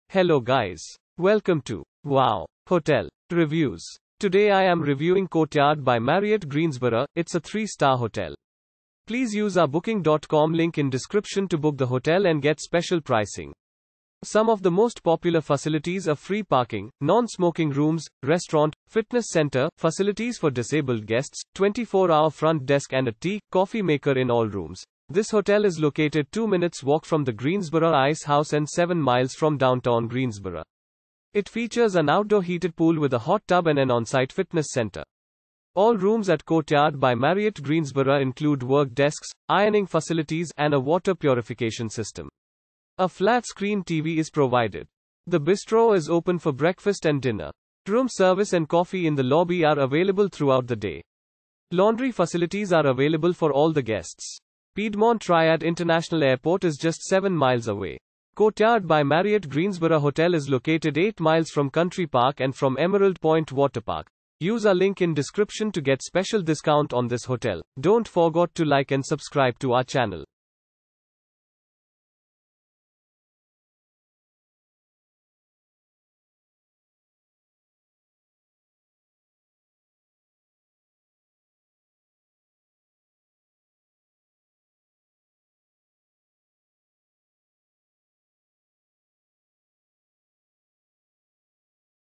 Courtyard by Marriott Greensboro Review - Greensboro , United States of America
Courtyard by Marriott Greensboro

This hotel is located 2 minutes' walk from the Greensboro Ice House and 7 mi from downtown Greensboro. It features an outdoor heated pool with a hot tub and an on-site fitness center.
All rooms at Courtyard by Marriott Greensboro include work desks, ironing facilities, and a water purification system. A flat-screen TV is provided.
The Bistro is open for breakfast and dinner. Room service and coffee in the lobby are available throughout the day. Laundry facilities are available for all the guests.
Piedmont Triad International Airport is just 7 mi away. Courtyard by Marriott Greensboro hotel is located 8 mi from Country Park and from Emerald Pointe Water Park.